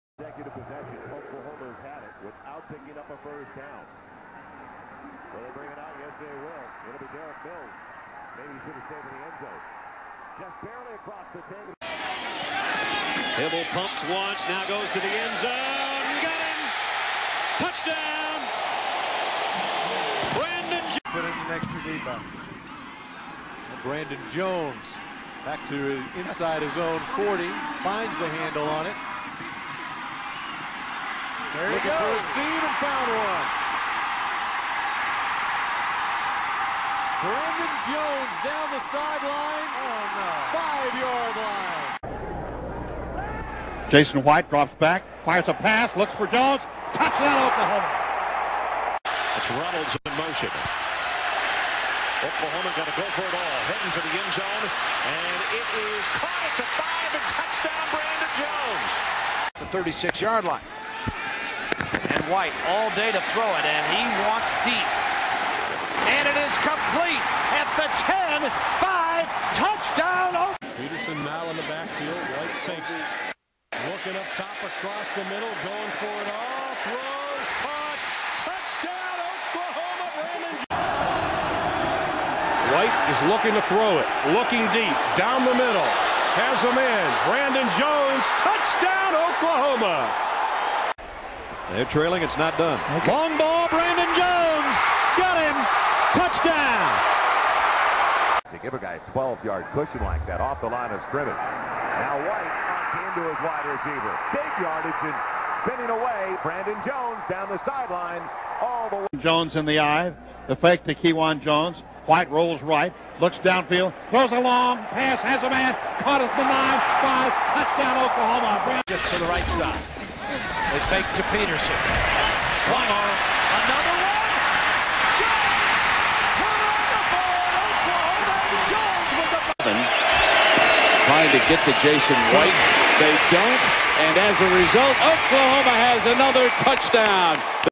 Brandon Jones highlights
Deep Threat specialist and 5 year NFL vet( are you listening St. Louis Rams?) Brandon Jones